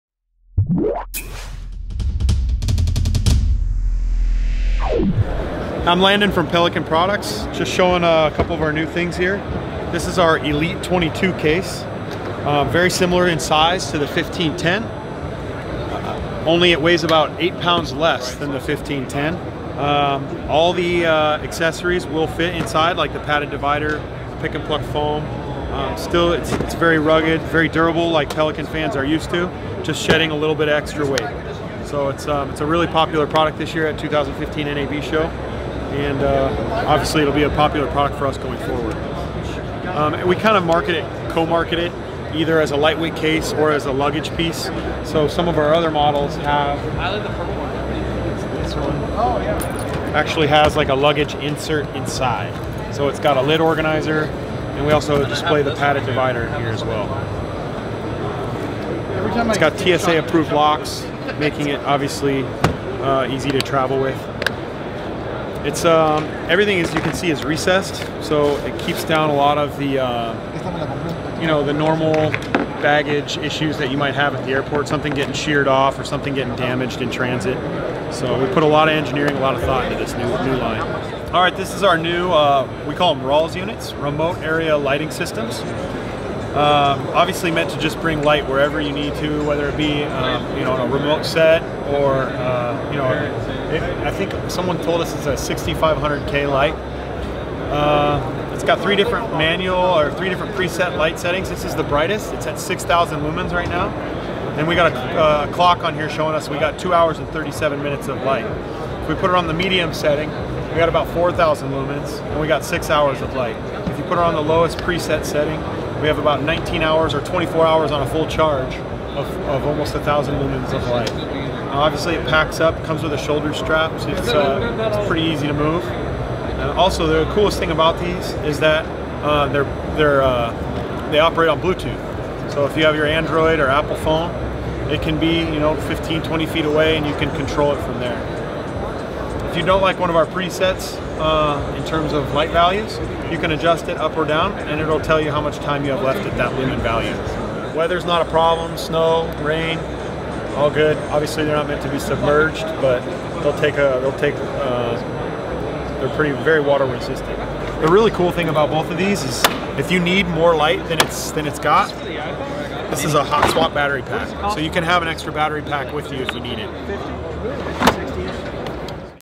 Pelican Elite 22 Case and RALS - NAB 2015
Landon from Pelican Products gives us a sweet overview of two very cool products from the line up.

Pelican reminds us to always use protection.